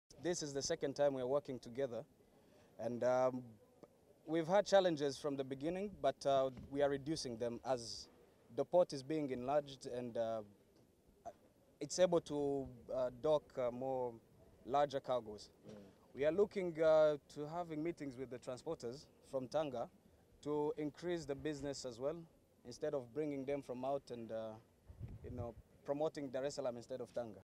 MAUAYO FABRICE:Meneja biashara wa kampuni ya Prime Cement ya nchini Rwanda.
Meneja biashara  wa  kampuni ya Prime Cement ya nchini Rwanda Muayo Fabrice akitoa shukrani kwa maboresho ya bandari ya Tanga  ambayo yatawawezesha kufanya biashara kupitia bandari hiyo.